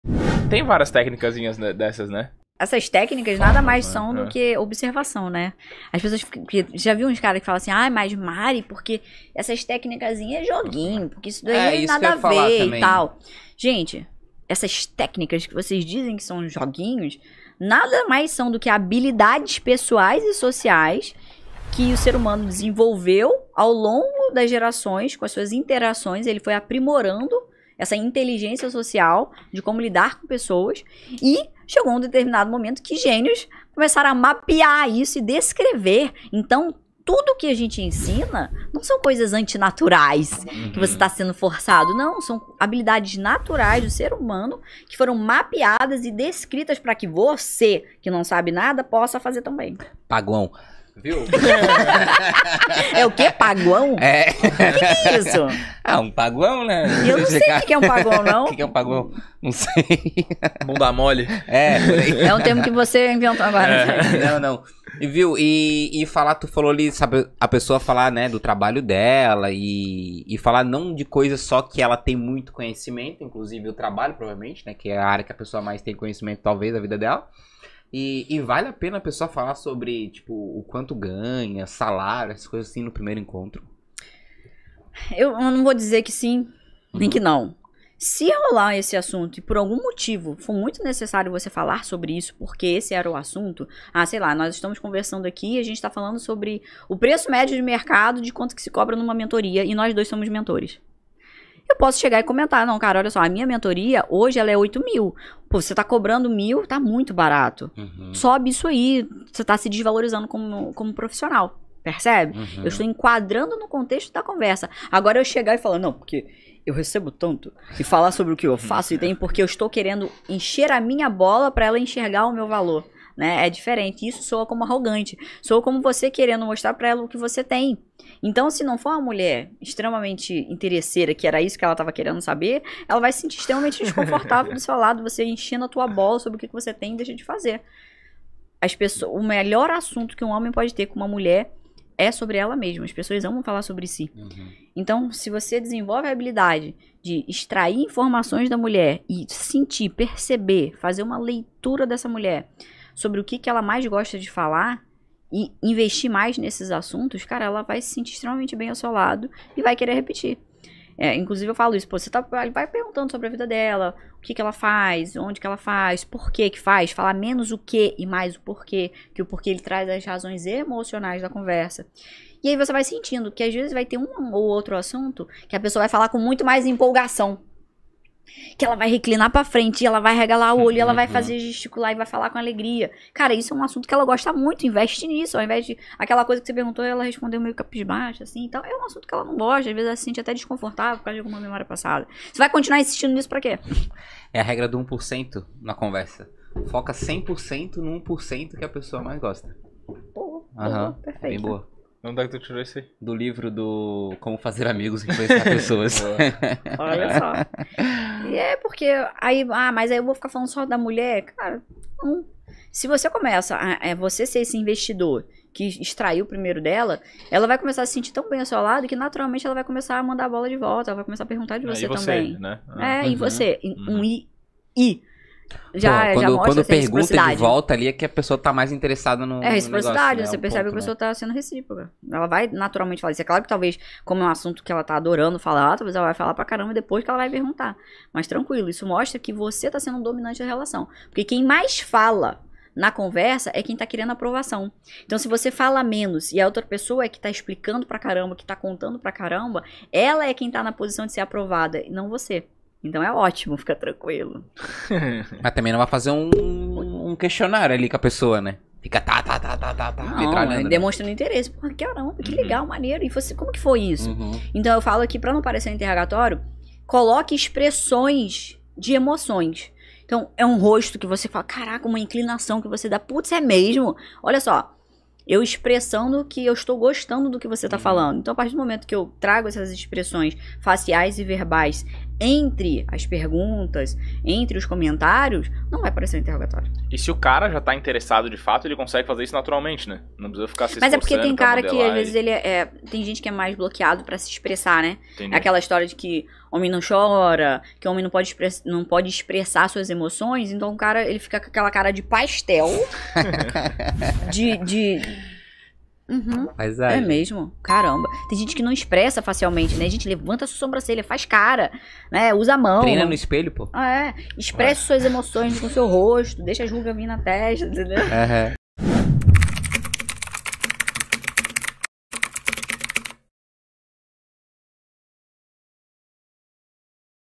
A TÉCNICA PODEROSA PARA TER ASSUNTO COM AS MULHERES | Mari Vabo
Tenha acesso a Netflix da conquista, mais de 20 horas de conteúdo | Acesse agora

Você já passou pela situação de se sentir sem assunto ao conversar com uma mulher? Não se preocupe! Mari Vabo está aqui para te ajudar a superar essa dificuldade. Neste vídeo, ela vai revelar uma técnica eficaz para manter uma conversa interessante e envolvente com as mulheres, seja em encontros românticos, festas ou até mesmo no dia a dia.

Ao longo do vídeo, Mari Vabo vai fornecer dicas práticas, exemplos reais e insights valiosos sobre como desenvolver assuntos interessantes e atraentes para conquistar a atenção das mulheres. Você aprenderá a criar conexões genuínas, explorar interesses mútuos e manter uma conversa cativante do início ao fim.

Se você deseja se destacar nas suas interações com as mulheres, ganhar confiança e construir relacionamentos significativos, não pode perder este vídeo. Aproveite essa oportunidade única de aprender com a experiente Mari Vabo e dominar a técnica poderosa para ter assunto com as mulheres.

Ative as notificações para não perder nenhum dos próximos vídeos repletos de dicas valiosas.